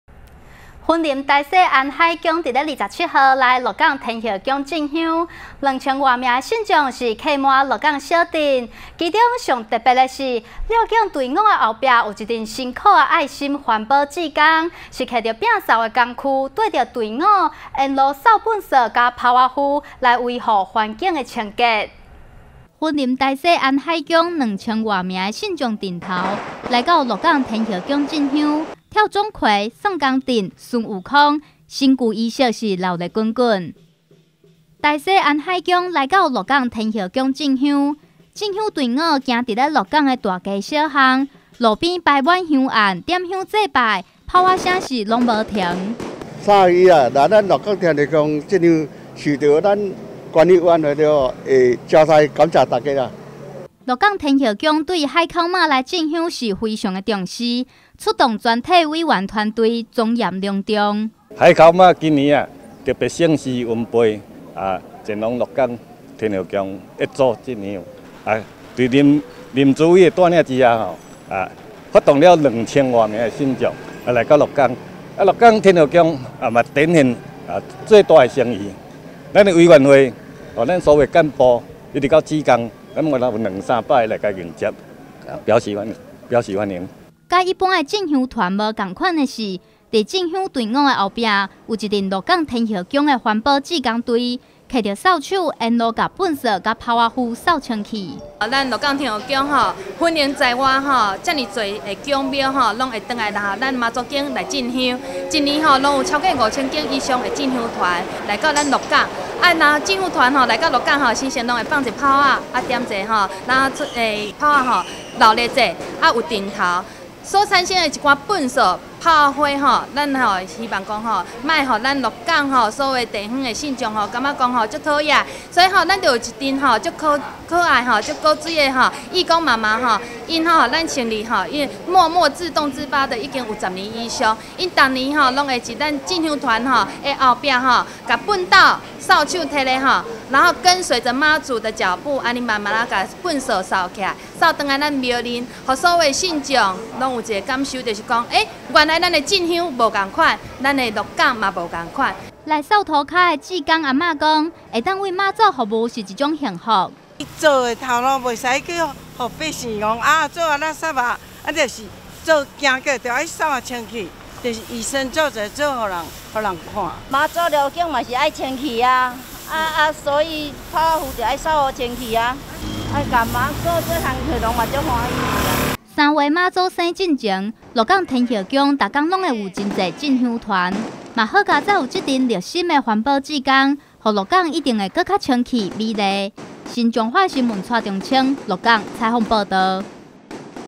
新彰化新聞0427 台西安海宮鹿港進香 志工沿路維護清潔
雲林台西安海宮在27日來鹿港天后宮進香，兩千多名信眾擠滿鹿港小鎮，其中最特別的是繞境隊伍後面，有一群辛苦的愛心環保志工，拿著清掃工具，跟著隊伍沿路清掃垃圾以及炮灰，維護環境的清潔。